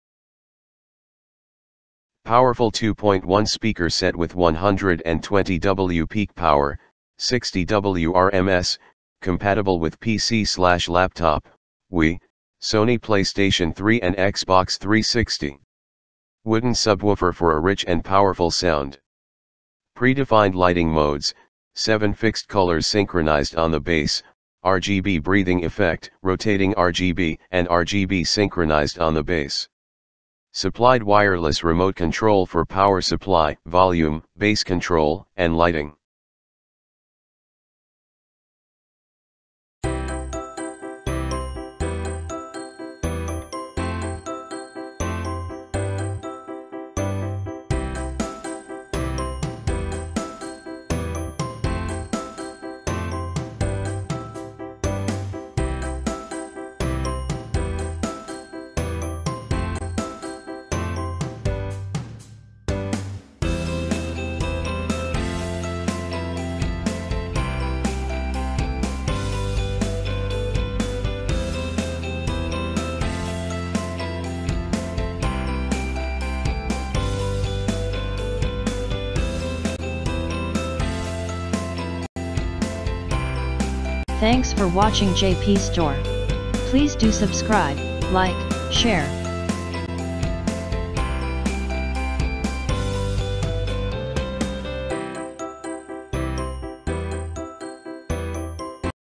Trust Tytan GXT 629 Set Altoparlanti 2.1 con Illuminazione Led RGB
Potente set altoparlanti 2.1 con potenza di picco 120 W (60 W RMS); compatibile con PC/laptop,Wii, Sony PlayStation 3 e Xbox 360
Subwoofer in legno per un suono ricco e potente
Modalità predefinite per l'illuminazione: 7 colori fissi sincronizzati sui bassi, RGB effetto breathing, RGB rotanti e RGB sincronizzati sui bassi
In dotazione telecomando wireless per alimentazione, volume, controllo di bassi e illuminazione